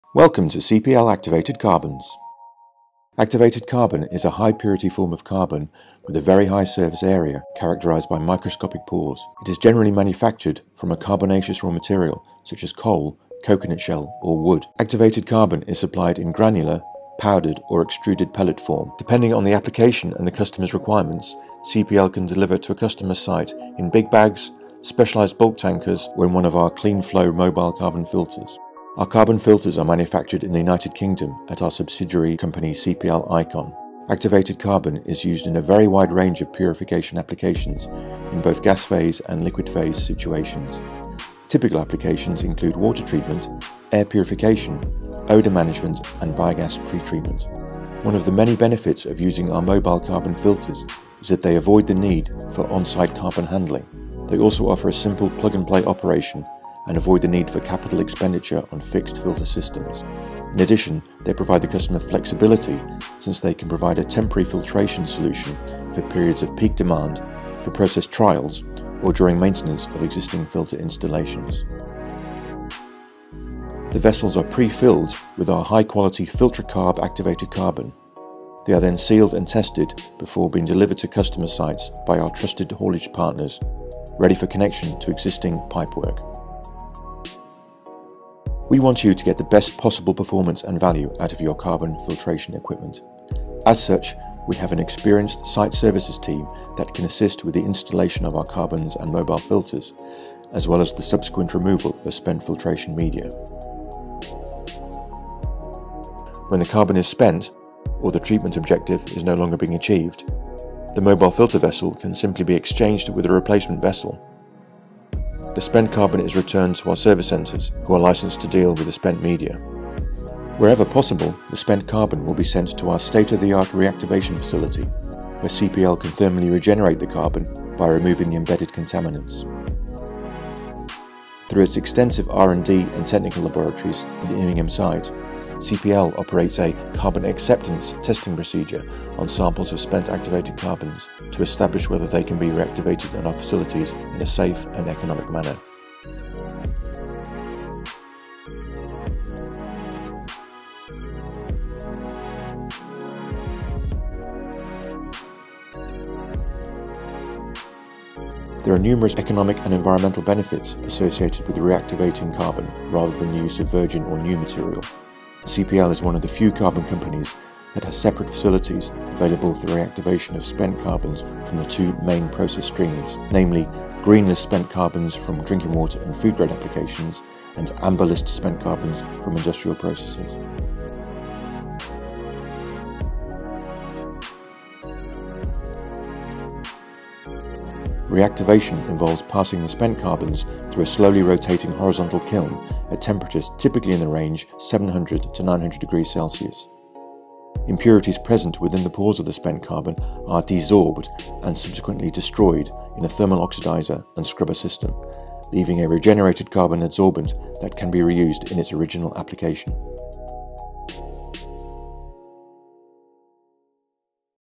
An Introduction to CPL Activated Carbons
CPL Activated Carbons is a leading manufacturer and supplier of activated carbons and related services and has enjoyed strong growth in the purification market thanks to a number of factors:

- Global sourcing capability, able to obtain the best carbons at competitive prices.
- Broad product range consisting of coal, coconut shell and wood-based carbons, sold under our Filtracarb® brand name.
- An active R&D programme, linked to our parent company CPL Industries, looking into new carbon materials and technologies.
- State-of-the-art reactivation facility, able to regenerate spent carbons from ‘green’ (food & drinking water) and ‘amber’ (industrial/environmental) process streams.
- Broad range of Clean-Flo® mobile carbon filters, for liquid and gas phase applications.
- Site Services team able to facilitate the installation of carbon/filters and the removal/changeout of spent media.
- An experienced and knowledgeable sales & marketing team backed up by a friendly and efficient office organisation.
- Manufacturing capabilities and multiple logistics hubs in the UK. Fast response and short lead-times.
- Efficient network of production, warehousing and logistics capabilities
Market-leading position in several key carbon applications.
- Full accreditation to the latest quality (ISO 9001:2015), environmental (ISO 14001:2015) and legislative (REACH) standards.